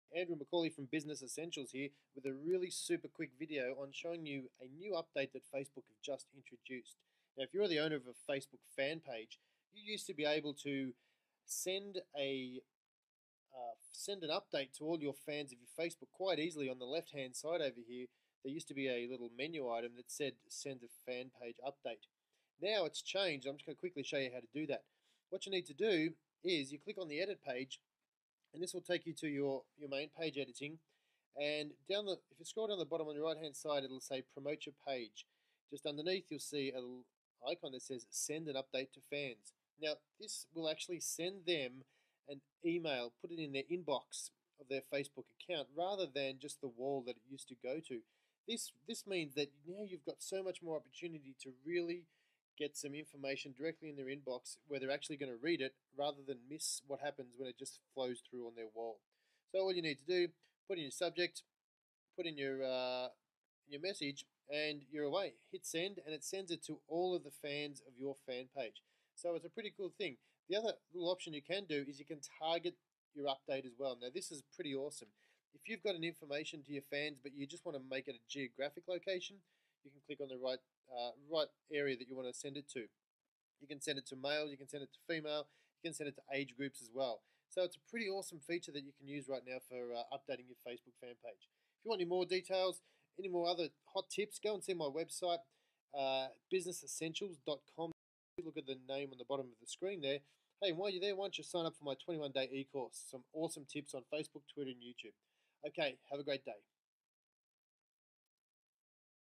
How To Send Facebook Page Updates
This video will show you how to send facebook page Updates with the new changes that Facebook have made to Fan pages. If you are an administrator for a facebook fan page, you should watch this video